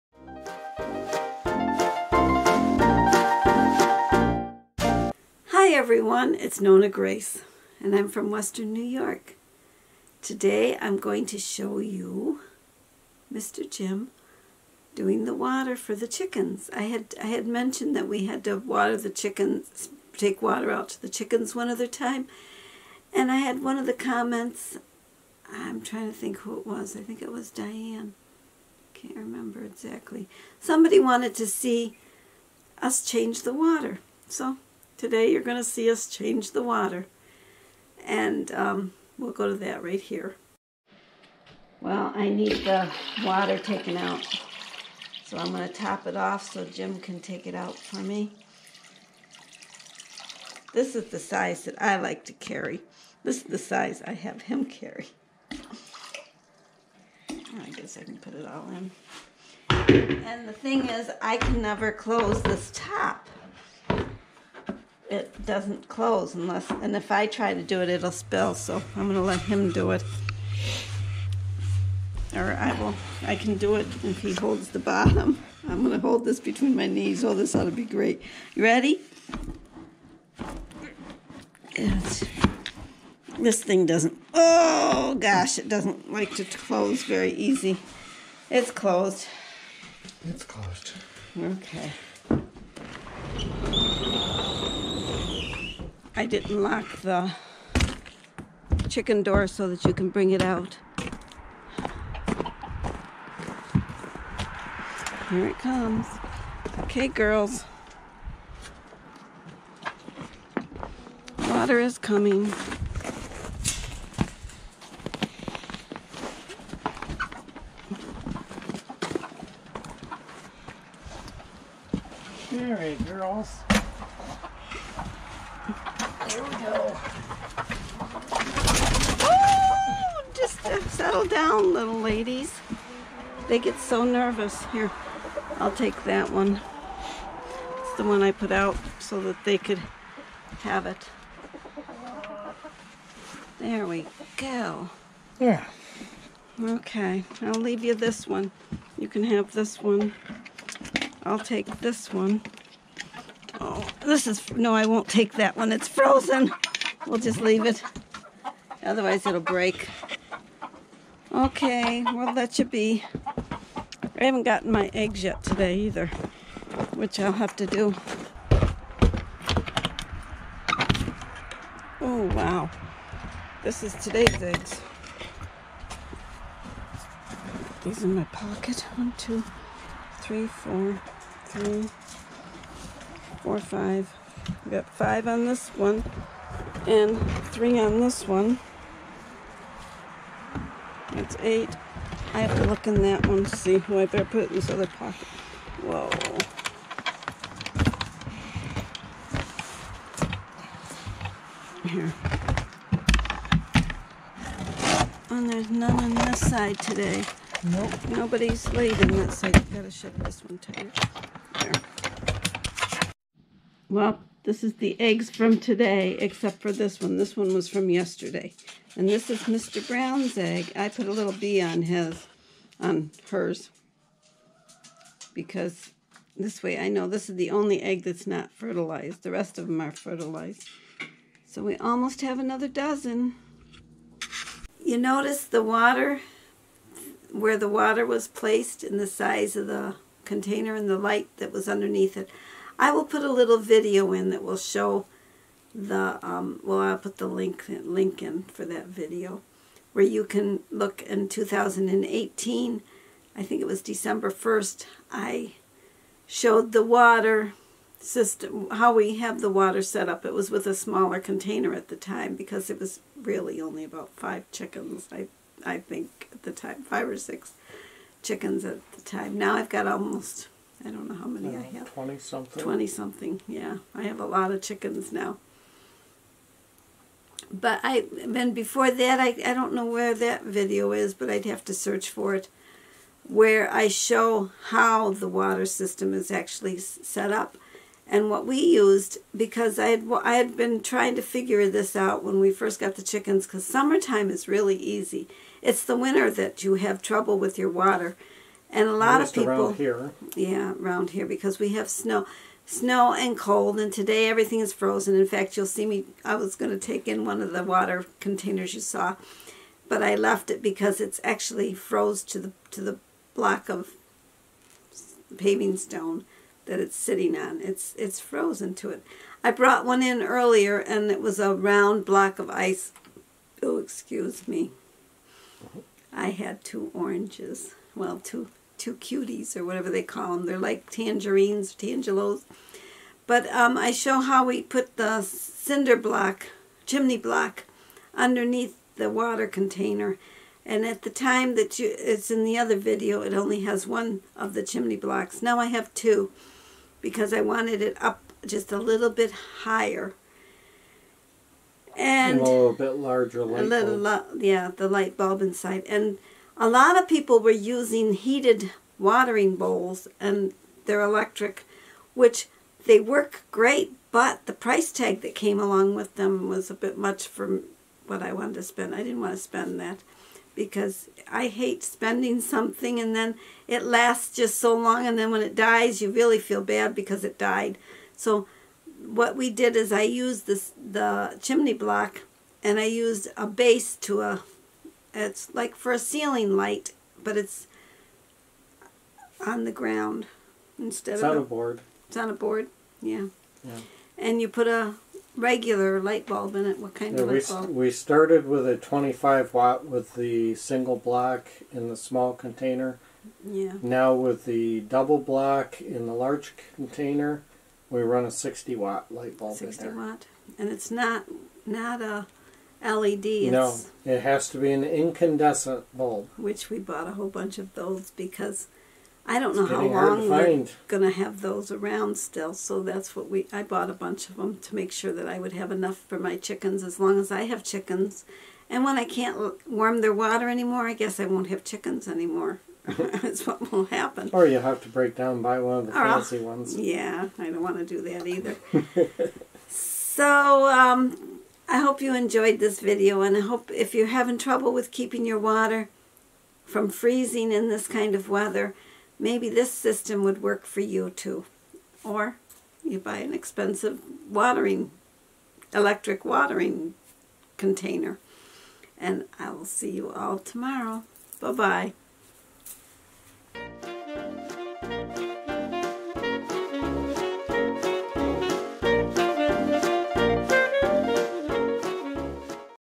Almost 3 Gal Of Water
The Chickens need their water changed so I had Jim bring out the Almost 3 gal of water container. It is too hard for me to close without spilling. almost 3 i like to fill the smaller container that is about one gal of water,

 I also talked about the water system that I have for my chickens so that their water doesn't freeze. The water system for chickens, that most people use is an electric one that costs a lot of money.
Here is the video where I show the water system in my chicken run.